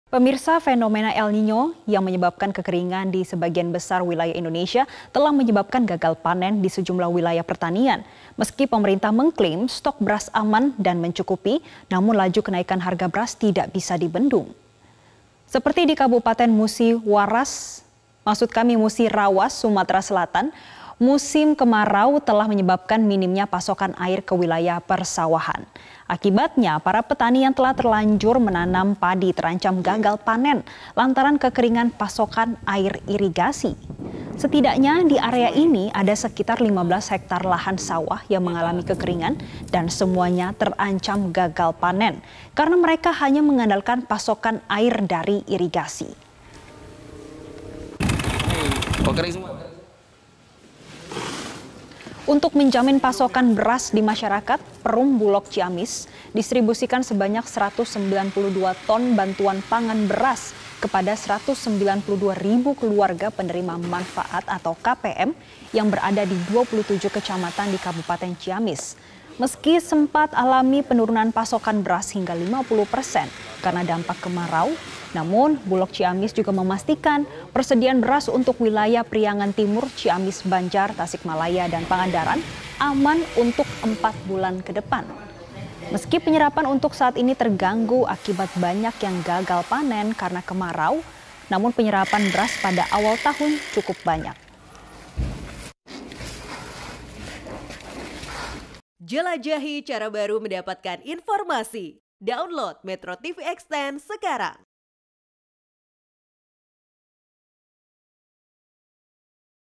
El Nino Picu Kekeringan Lahan, Petani Gagal Panen
MetroTV, Fenomena Elnino yang menyebabkan kekeringan di sebagian besar wilayah Indonesia, menyebabkan gagal panen di sejumlah wilayah pertanian. Meski stok beras aman dan mencukupi namun laju kenaikan harga beras tidak bsa di bendung.